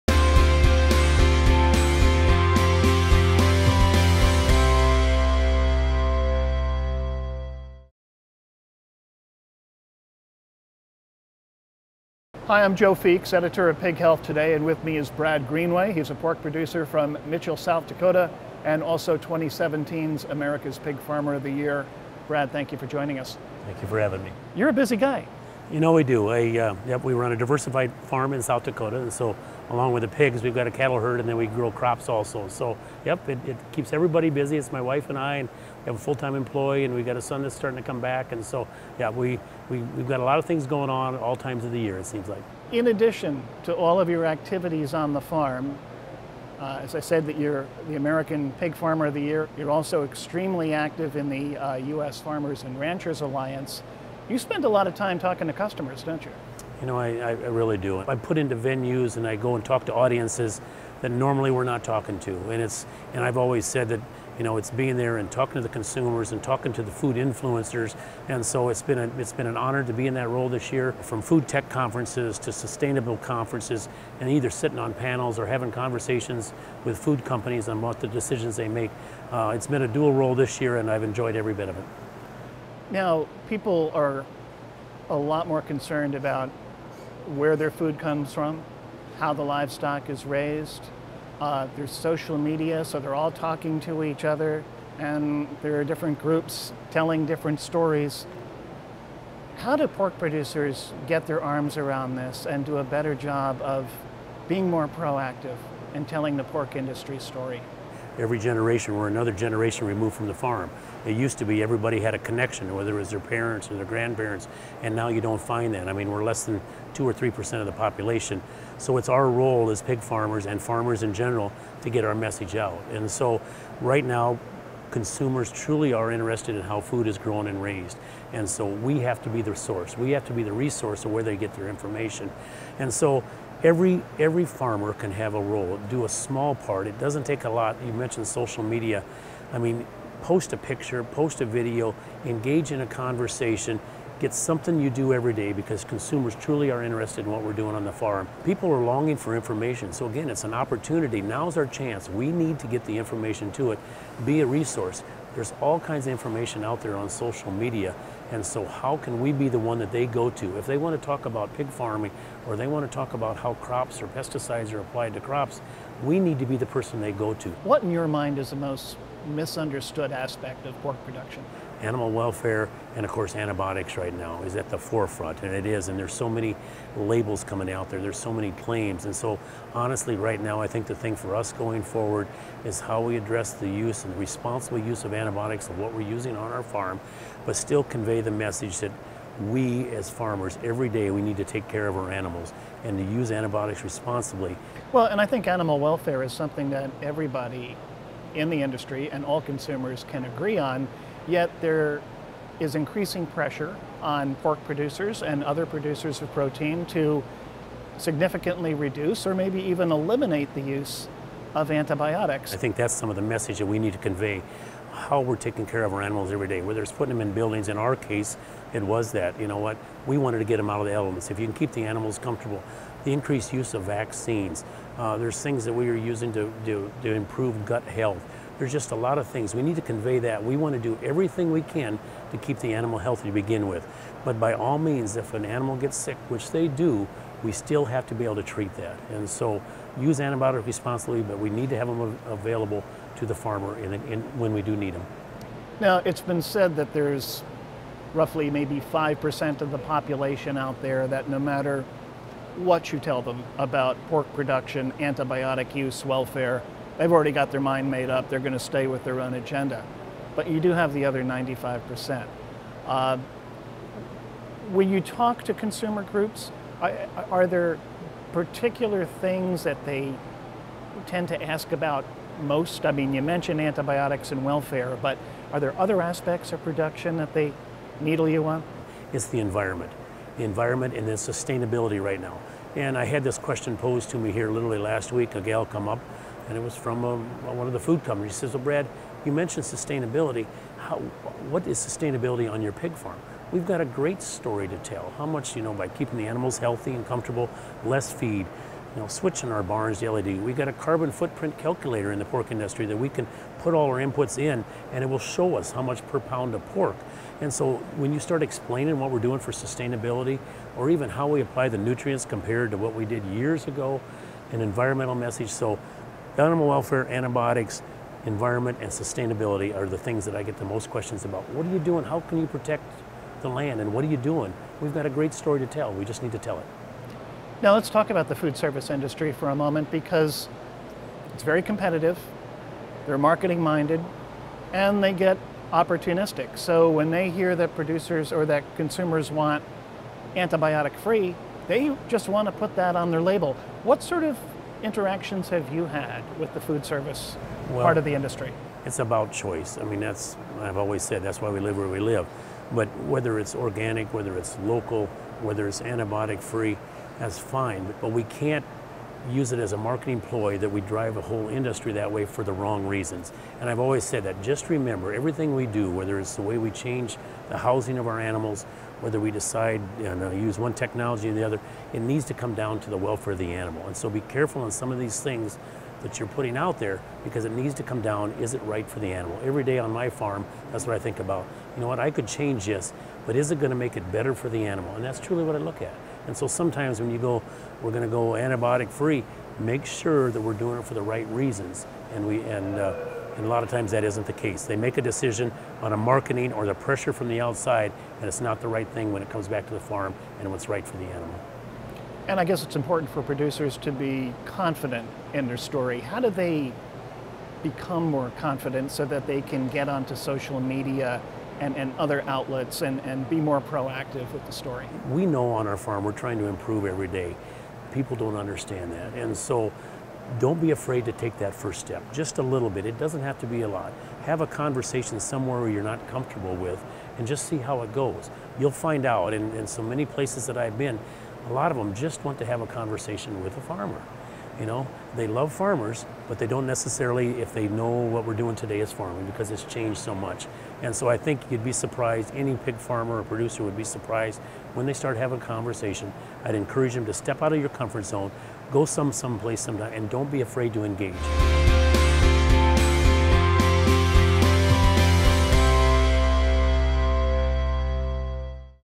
Talk with People About Animal Welfare and Responsible Antibiotic Use with Brad Greenway
Interview with Brad Greenway, the 2017 America's Pig Farmer of the Year, for Pig Health Today on the topic of stepping out of your comfort zone and talking to people about animal welfare and responsible antibiotic use. Interview conducted at World Pork Expo 2017.

To learn more about Pig Health Today, please visit pighealthtoday.com